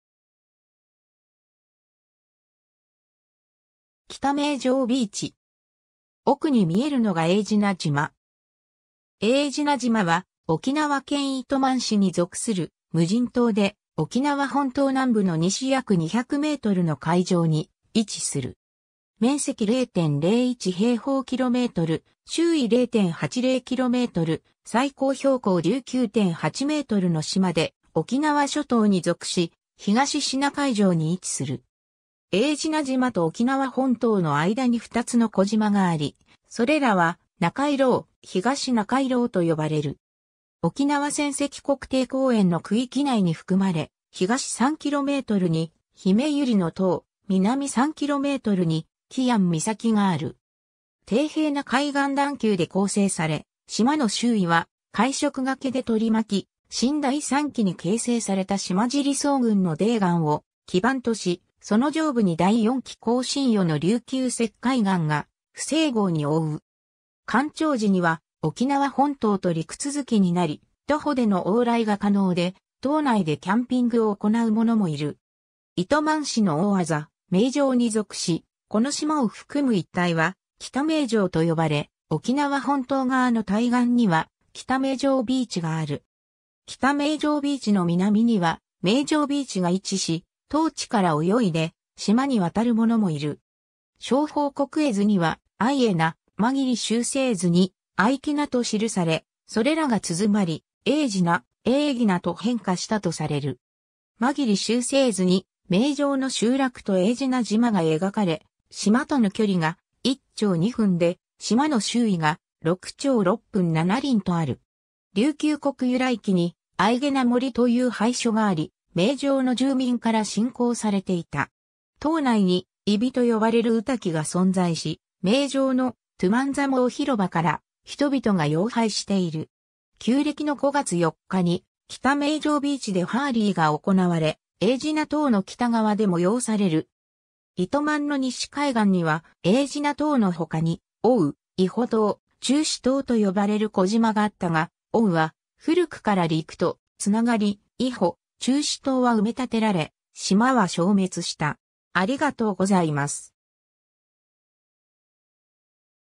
エージナ島
北名城ビーチ。
奥に見えるのがエージナ島。
 エージナ島（エージナじま）は、沖縄県糸満市に属する無人島で、沖縄本島南部の西約200メートルの海上に位置する。
 面積0.01平方キロメートル、周囲0.80キロメートル、最高標高19.8メートルの島で、沖縄諸島に属し、東シナ海上に位置する。
エージナ島と沖縄本島の間に二つの小島があり、それらは「ナカイロー」、「東（あがり）ナカイロー」と呼ばれる。
沖縄戦跡国定公園の区域内に含まれ、東3キロメートルにひめゆりの塔、南3キロメートルに喜屋武岬がある。
低平な海岸段丘で構成され、島の周囲は海食崖で取り巻き、新第三紀に形成された島尻層群の泥岩を基盤とし、その上部に第四紀更新世の琉球石灰岩が不整合に覆う。
干潮時には沖縄本島と陸続きになり、徒歩での往来が可能で、島内でキャンピングを行う者もいる。
糸満市の大字「名城（なしろ）」に属し、この島を含む一帯は「北名城」と呼ばれ、沖縄本島側の対岸には北名城ビーチがある。
北名城ビーチの南には名城ビーチが位置し、当地から泳いで島に渡る者もいる。
 『正保国絵図』には「あいえな」、『間切集成図』に「あいきな」と記され、それらがつづまり「エージナ」、「エーギナ」と変化したとされる。
『間切集成図』に名城の集落とエージナ島が描かれ、島との距離が「一町二分」で、島の周囲が「六町六分七厘」とある。
『琉球国由来記』に、「アイゲナ森（エージナウタキ、エージナムイ）」という拝所があり、名城の住民から信仰されていた。
島内に「イビ」と呼ばれる御嶽が存在し、名城の「トゥマンザモー（十万座毛）広場」から人々が遥拝している。
旧暦の5月4日に、北名城ビーチでハーリーが行われ、エージナ島の北側で催される。
糸満の西海岸には、エージナ島の他に「奥武（おー）」・「伊保島」・「沖之島」と呼ばれる小島があったが、奥武は古くから陸とつながり、伊保・沖之島は埋立てられ、島は消滅した。